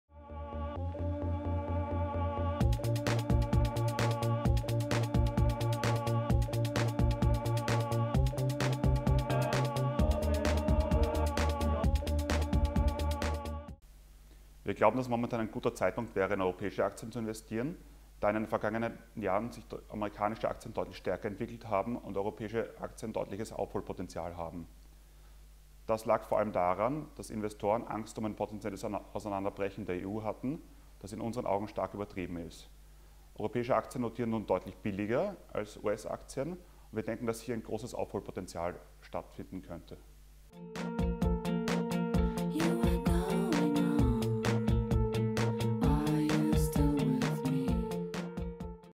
WIENER PRIVATBANK: Chancen am europäischen Aktienmarkt
Florian RAINER, Fondsmanager bei MATEJKA & PARTNER ist Teil des Experten-Teams der WIENER PRIVATBANK